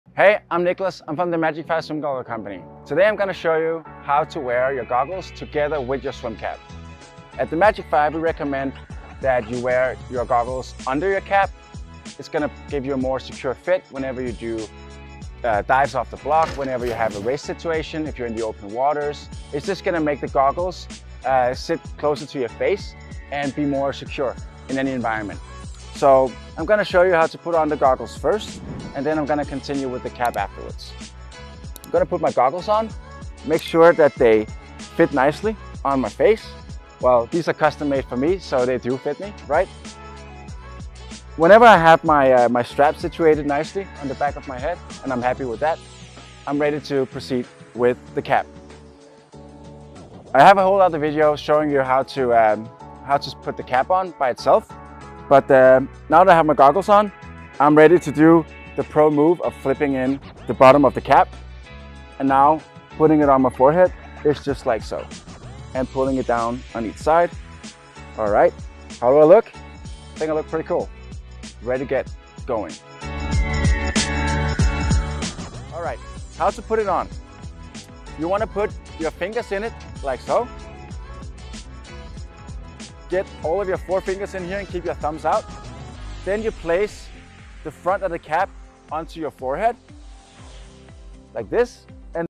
Swim Gear Mastery: Goggles & Cap - The Pro's Playbook!
🏊‍♀️ Elevate your swim game with this essential playbook by Niklas. Learn the art of wearing swim goggles and cap like a seasoned pro. Niklas showcases his technique for hassle-free adjustments and perfect fits. Whether you're a competitive swimmer or a casual enthusiast, this guide is a must-watch for taking your swimming performance to the next level!

00:00 - Intro
00:09 - THEMAGIC5 recommends
00:26 - Put on the Swim Goggles
00:47 - Put on the Swim Cap